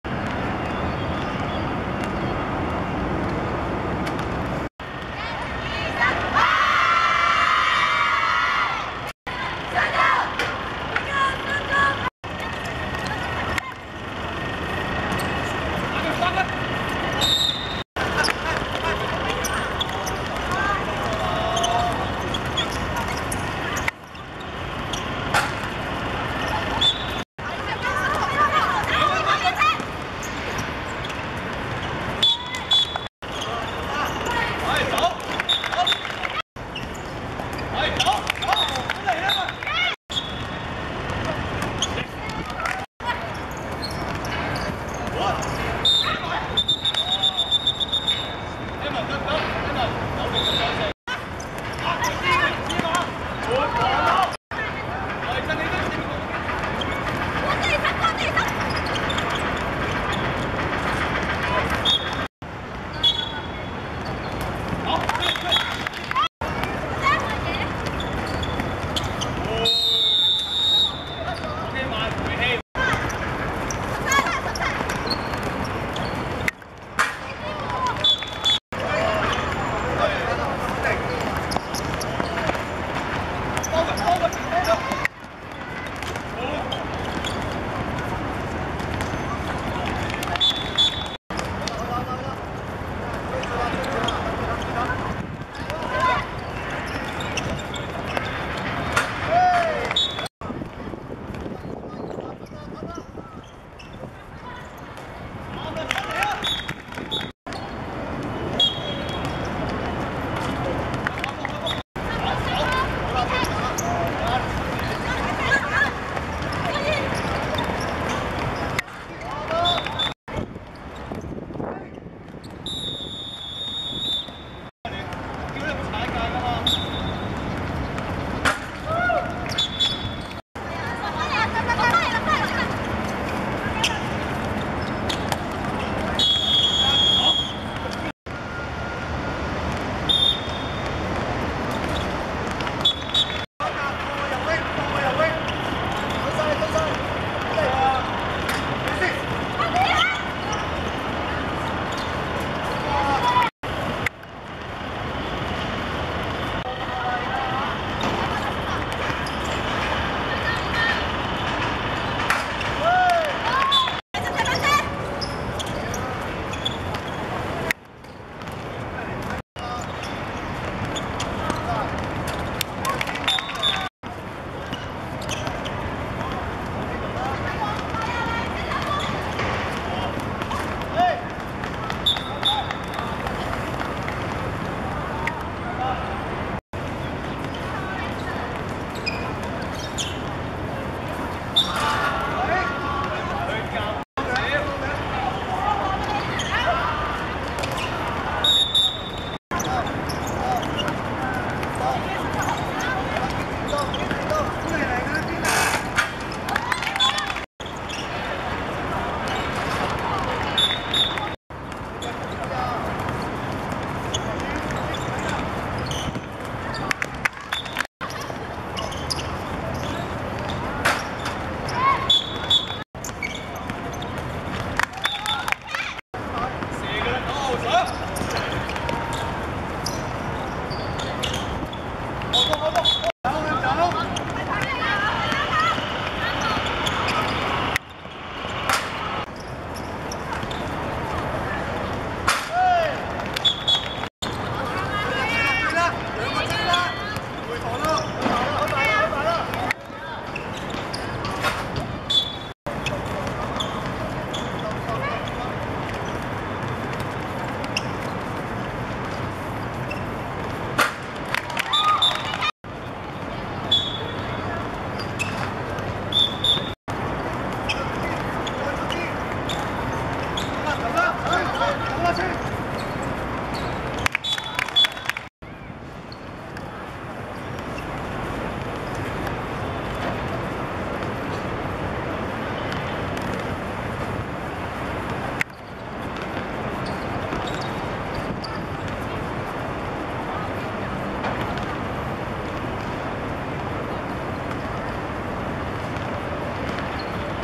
元朗商中vs聖傑靈(2014.1.14.學界女子手球精英賽八強)精華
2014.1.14.彩虹運動塲 17:00 元朗商中(白) 18:10 聖傑靈(紅)